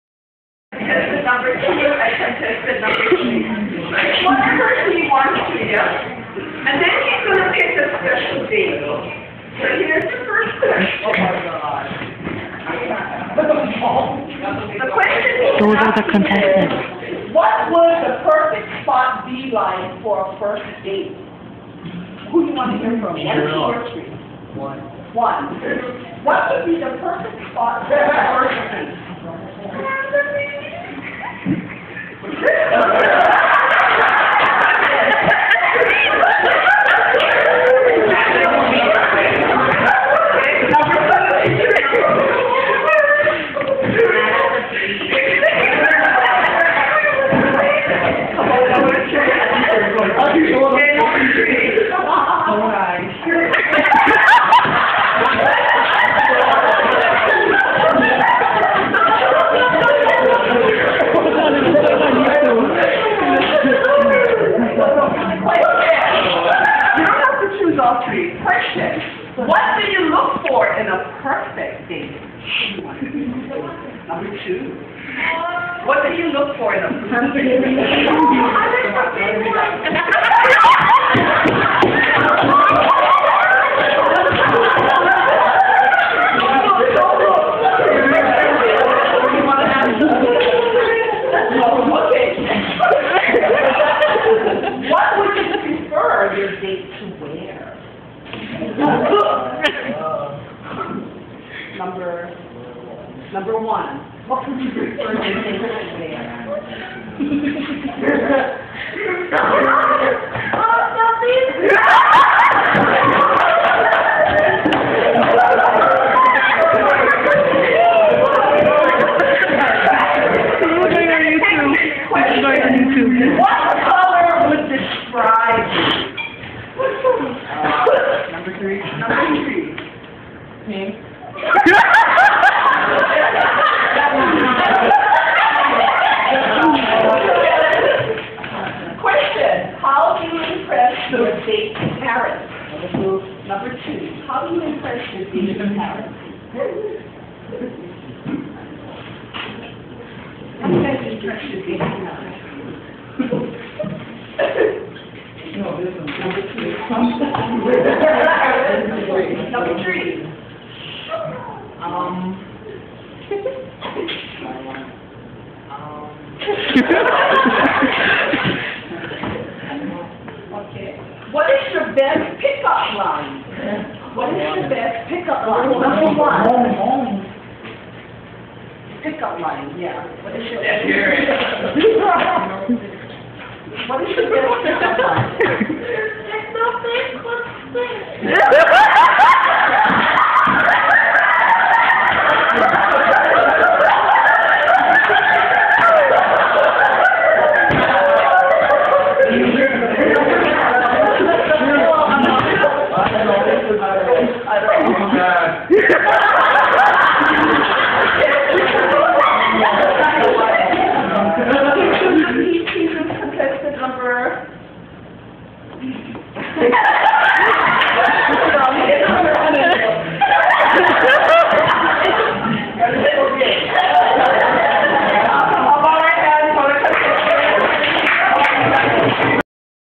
Waianae Boys
Boys In My Health Class Playing "Blind Date" The Bachelor Has NO Clue Who He's Up For. Lol